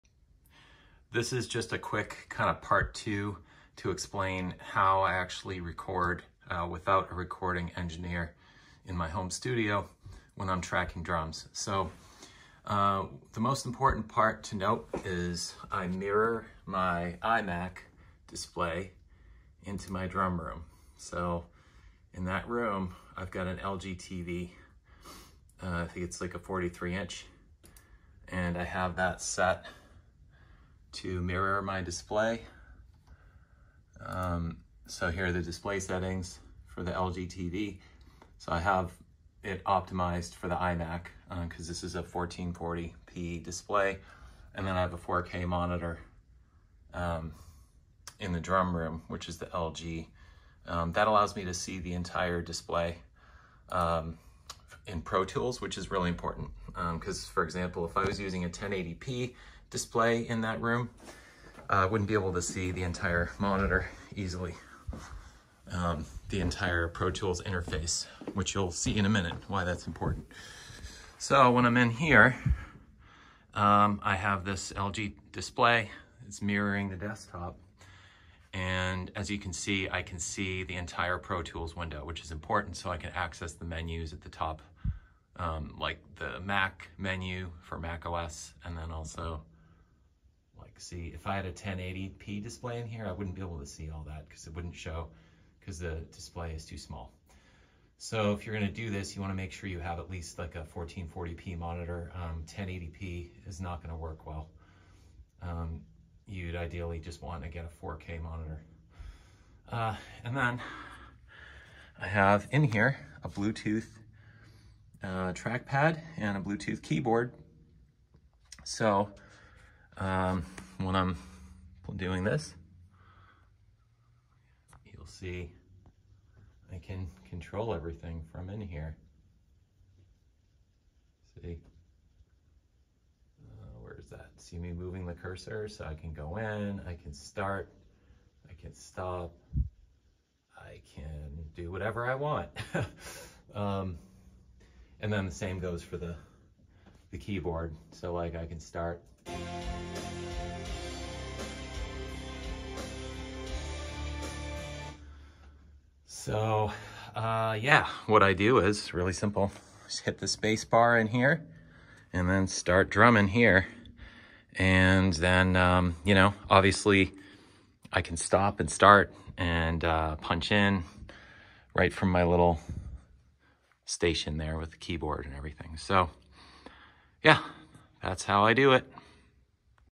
Part 2: Tracking drums as the drummer in Pro Tools without an engineer
Part 2: How to mirror your Mac display in your drum room ￼and start and stop your recording remotely.